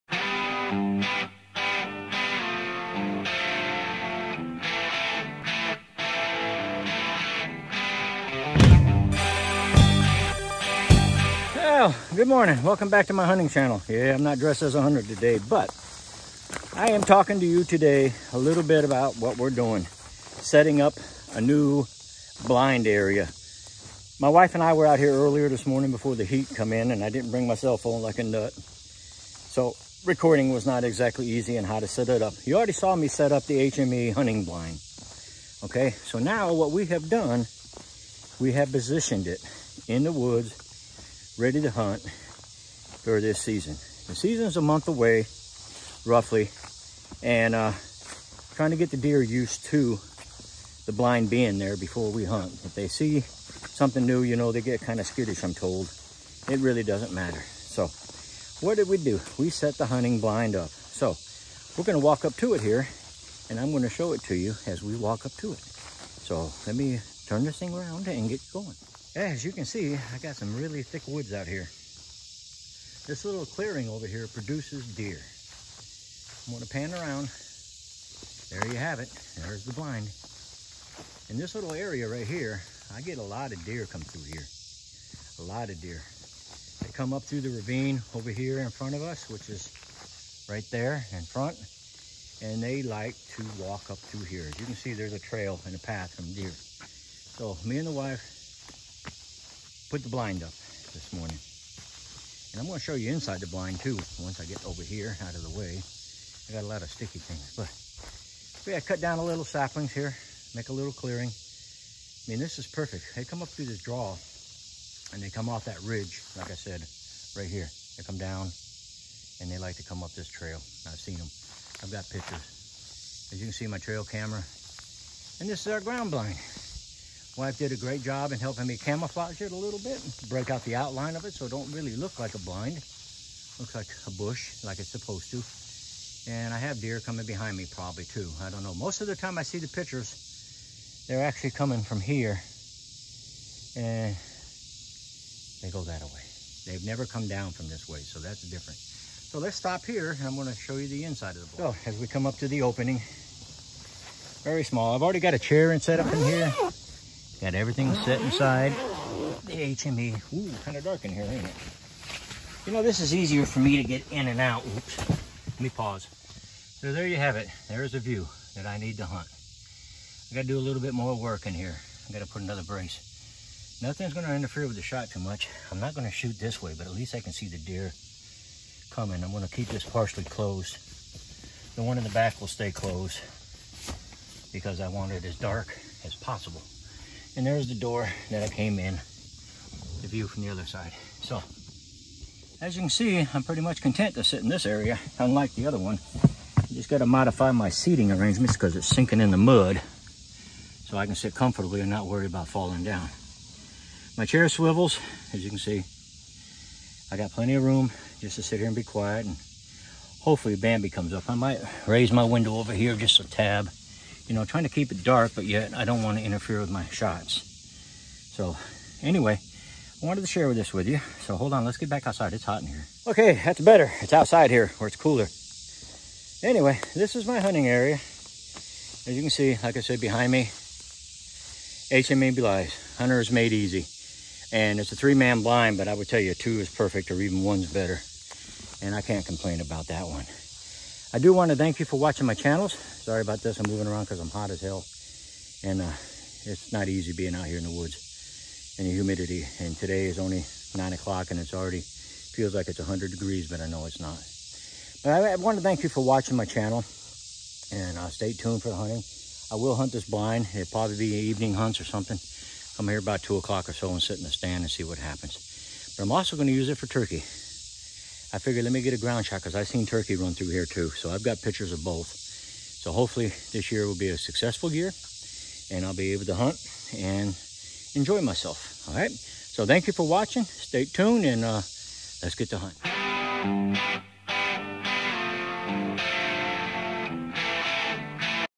My Hunting 2021 " HME Blind update and in Place"
Well hunting is near so the wife and I put the blind up preparing to hunt. It did take both of us as I somehow could not get it to pop open. We cut trees and placed them around it to break it up